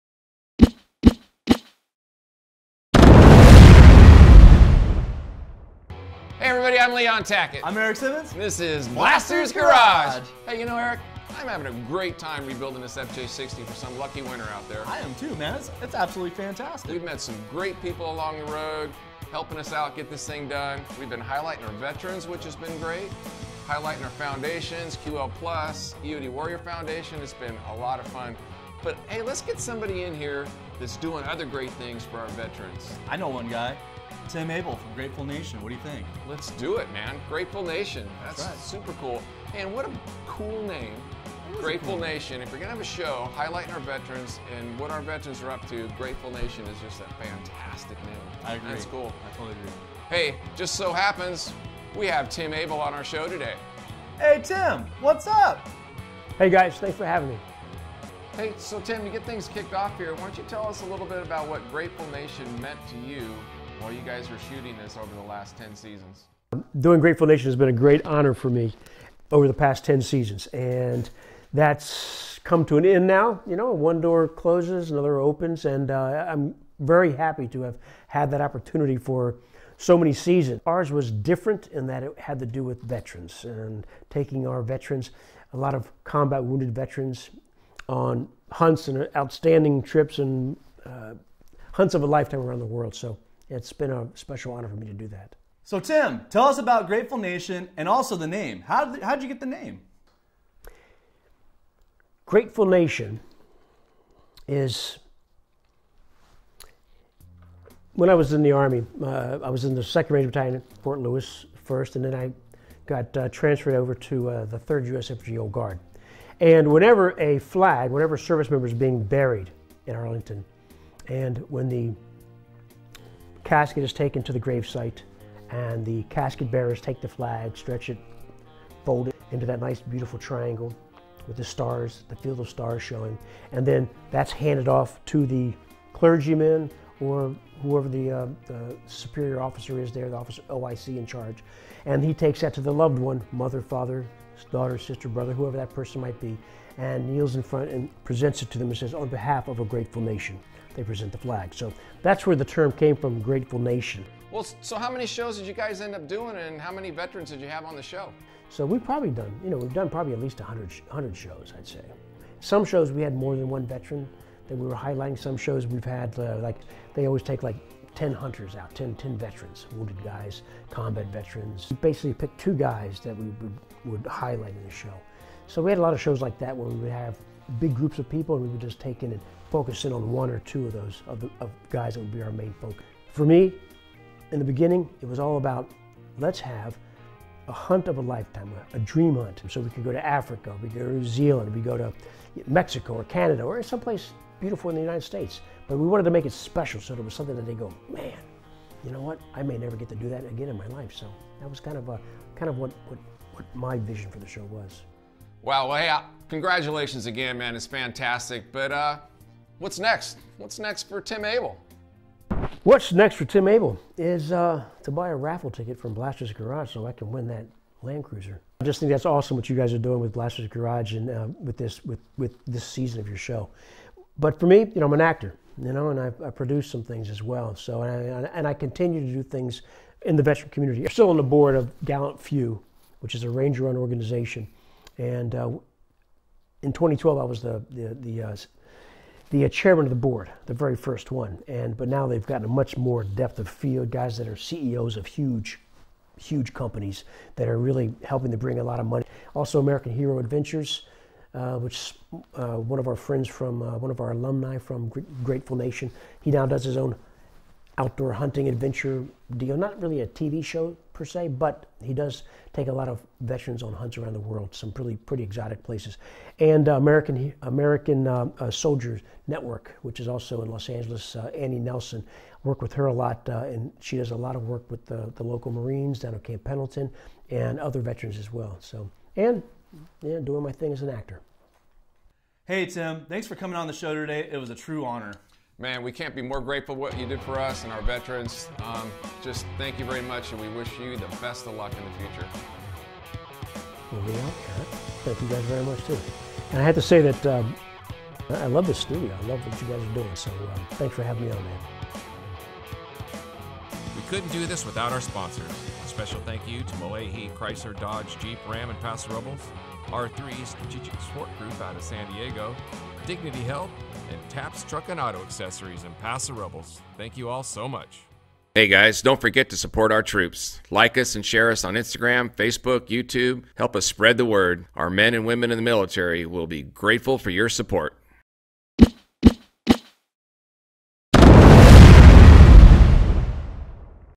Tim Abell - Grateful Nation
Check out our interview with Tim Abell, host of the Outdoor Channel show called, Grateful Nation. Tim just finished filming their 10th season, and it's on the Outdoor Channel now. Watch as he takes our veterans on some fantastic hunts.

As always - don't forget to checkout Blastersgarage.com to get your tickets to win a customized FJ60.